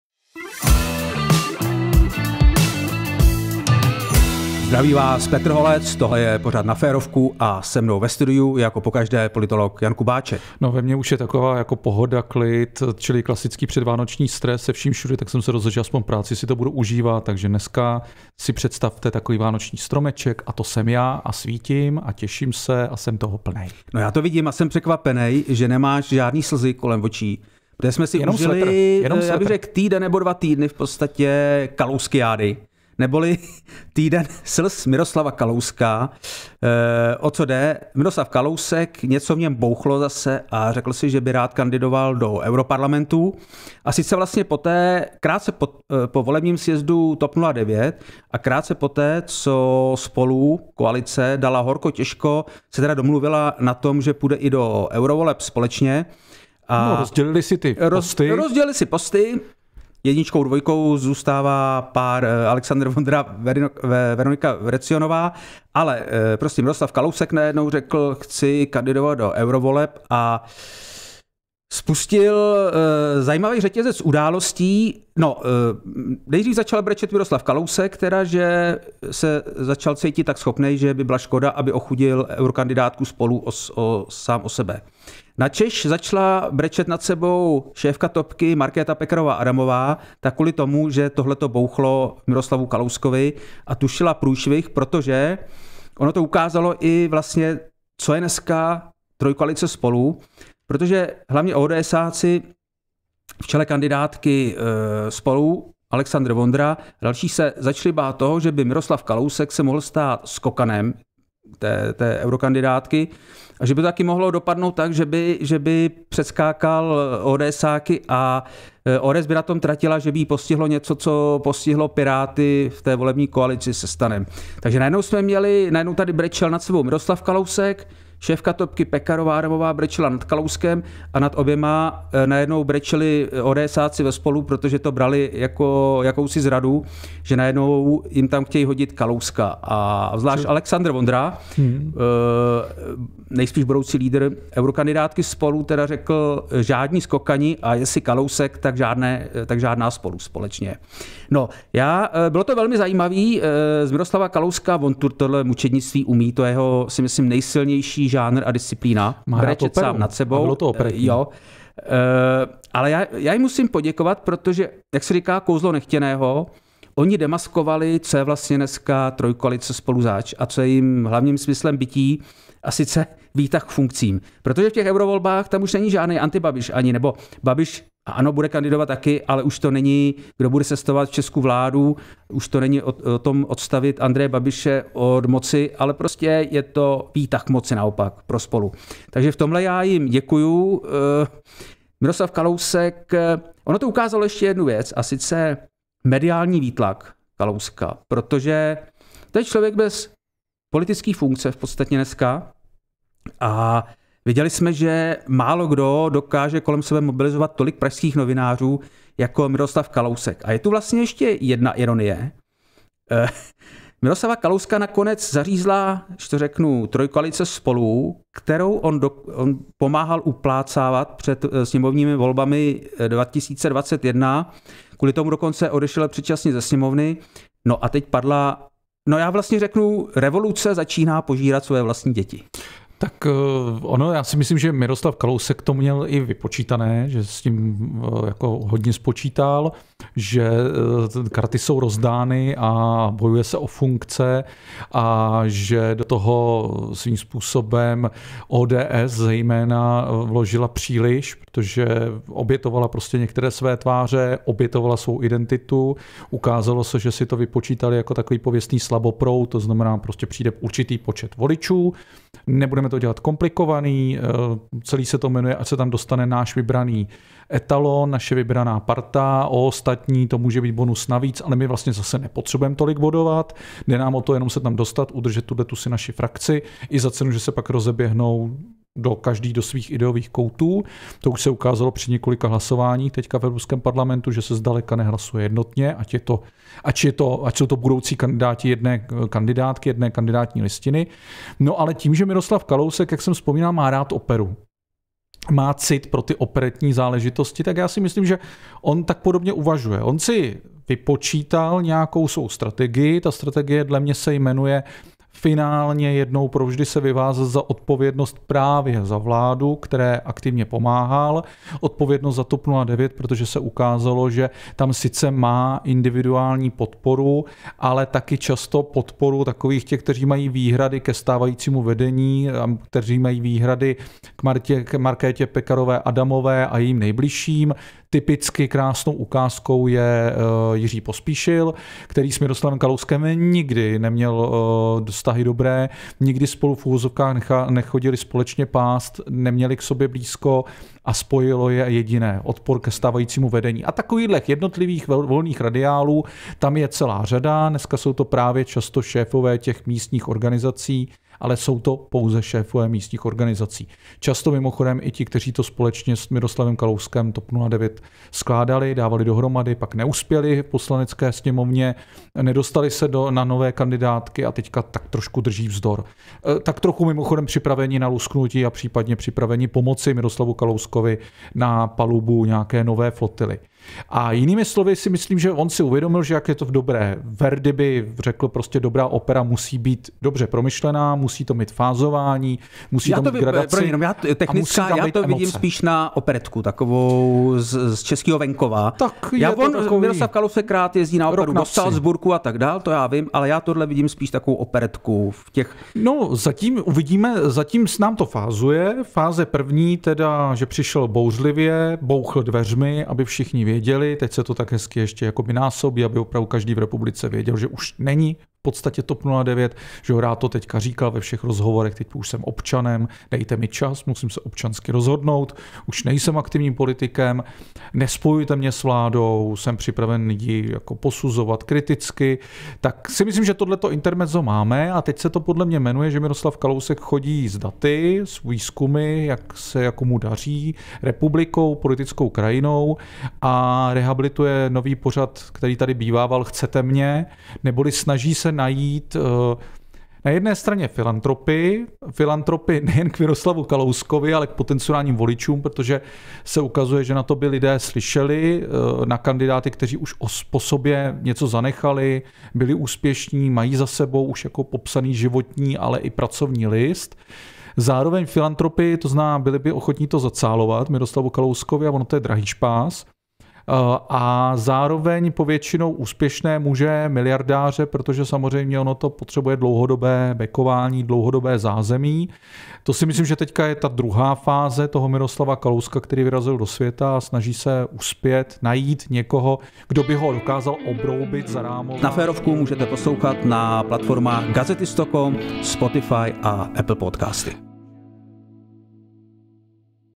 Chcete mě? Kalousek zesměšnil sebe, TOP09 i SPOLU
Petr Holec vám přináší pravidelný politický účet jako nikdo jiný, tentokrát s politologem Janem Kubáčkem.

Toto dílo podléhá licenci Creative Commons Uveďte původ-Neužívejte komerčně-Nezpracovávejte 4.0 Mezinárodní License.